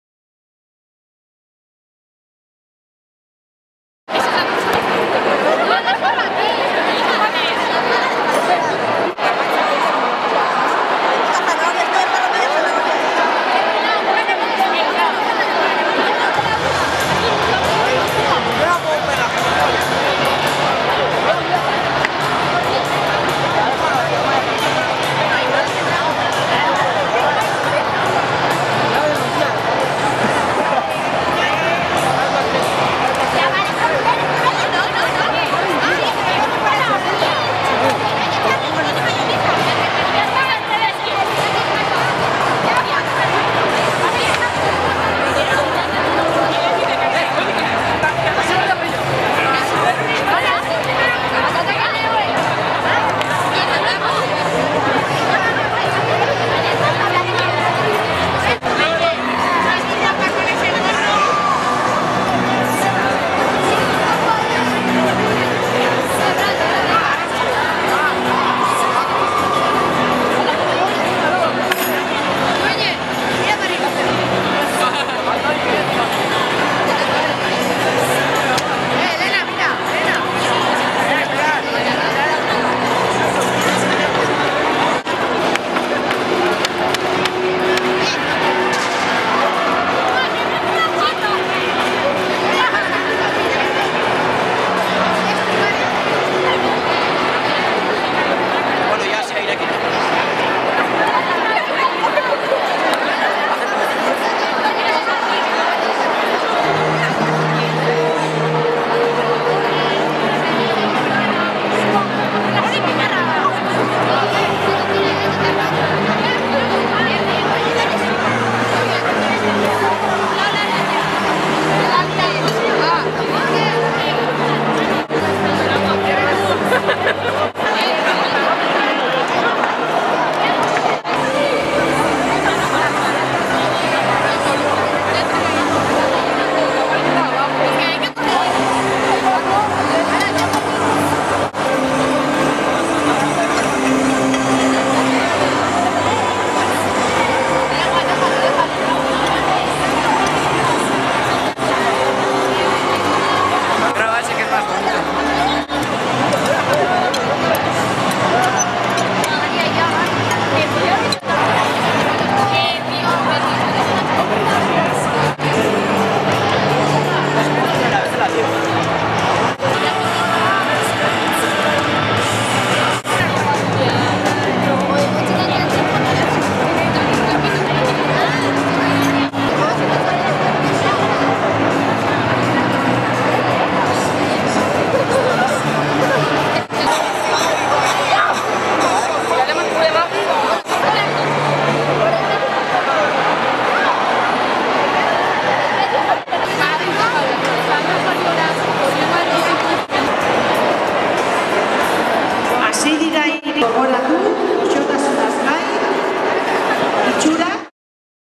Gazteen sukaldaritza lehiaketa. 2011 azaroa
Gazteen sukaldaritza lehiaketa jokatu da Ederrena pilotalekuan. 2011 azaroa